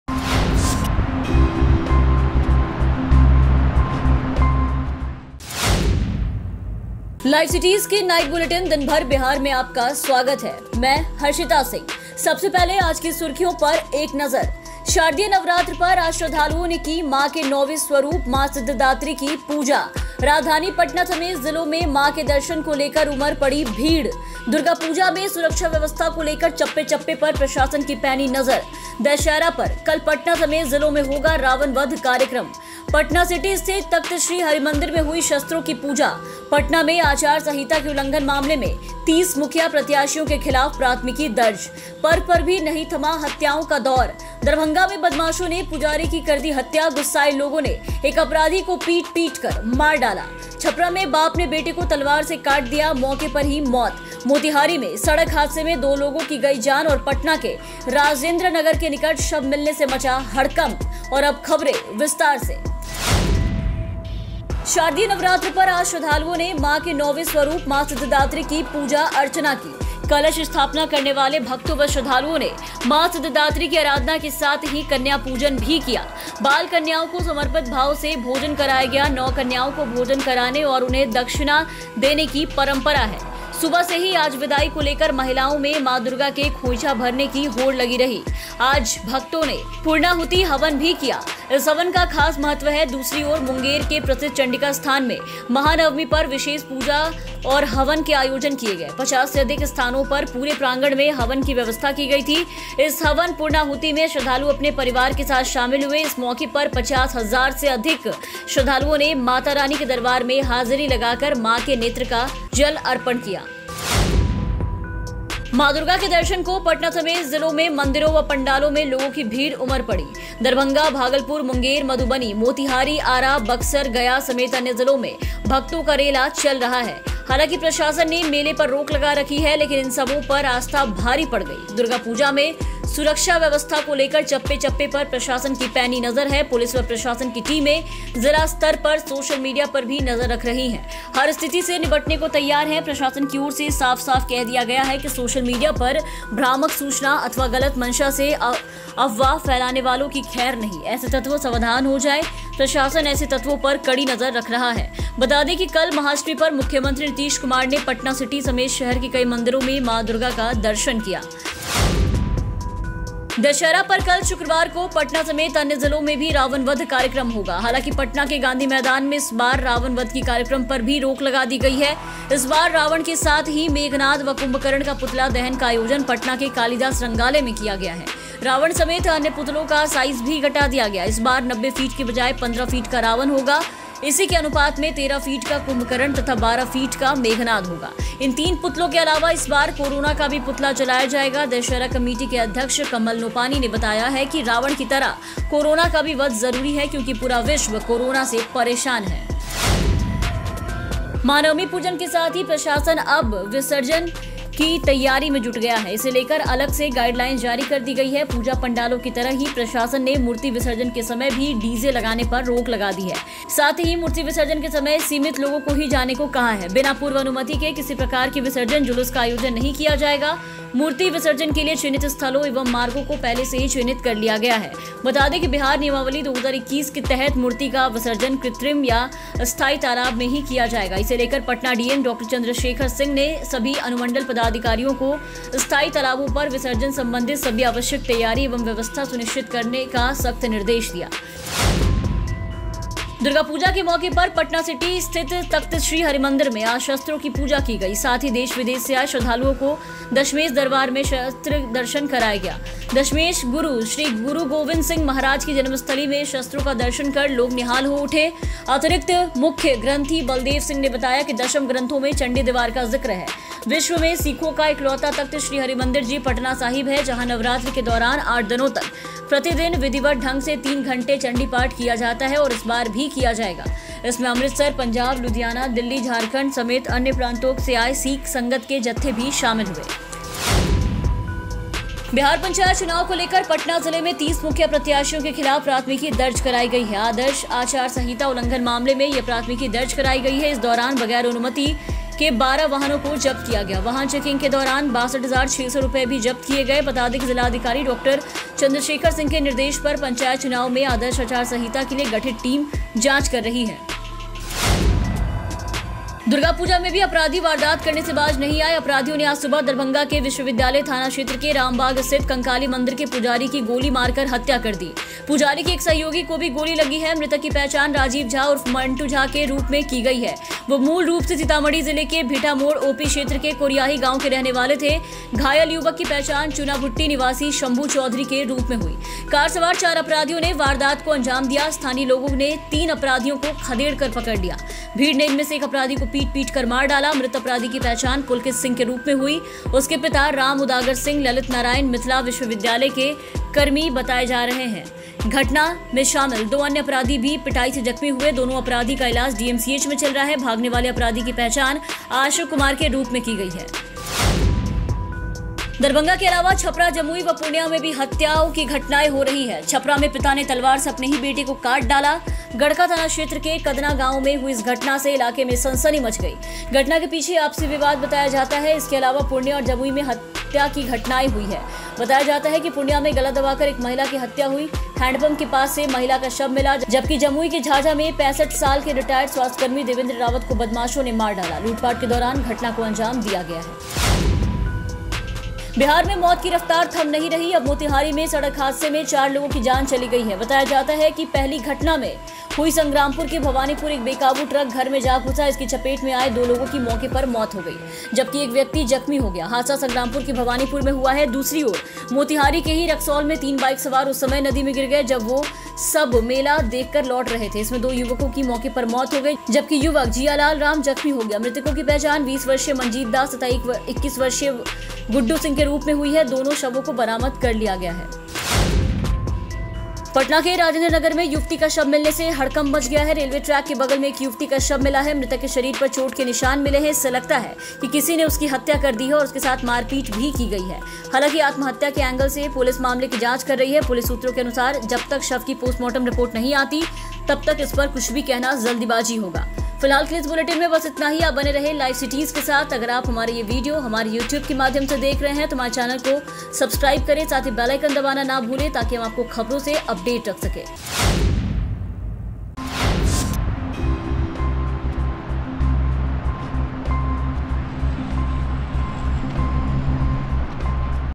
Durga Puja की Bihar में धूम, माता के दर्शन को उमड़ी भीड़, कल रावण वध, 30 मुखिया पर मामला दर्ज
Durga Puja की Bihar में धूम, माता के दर्शन को उमड़ी भीड़, कल रावण वध, 30 मुखिया पर मामला दर्ज | Bihar News | LiveCities